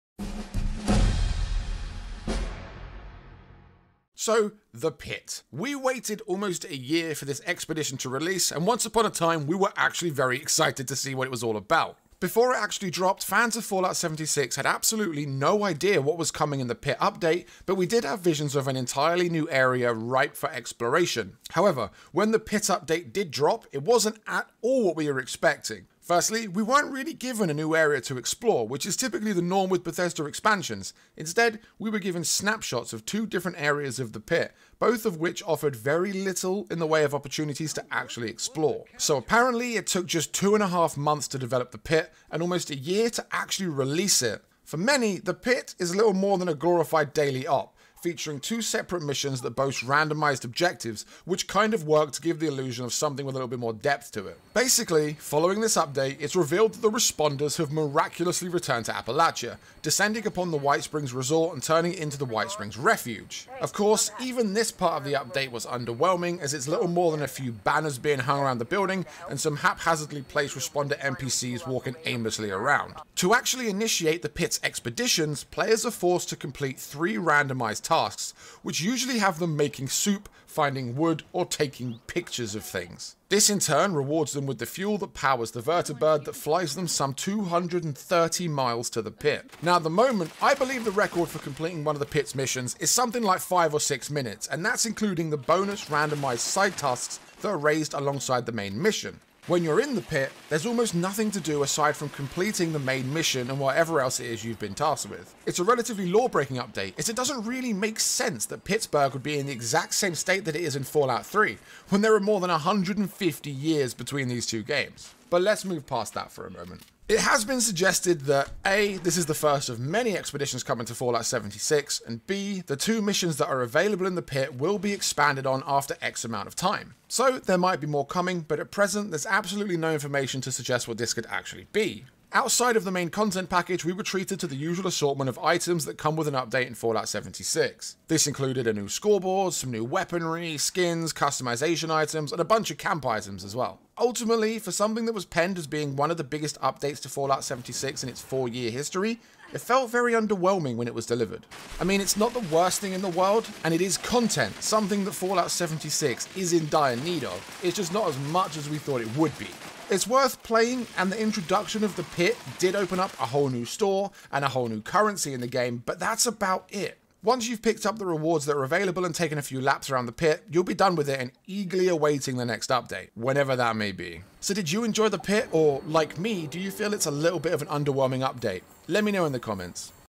Fallout 76 The Pitt Review (Quick Review) | Is The Pitt Good?
Is The Pitt any good, or is it a super disappointing update for Fallout 76? Find out in this super-short review that uncovers everything you need to know.

Fallout 76 The Pitt Review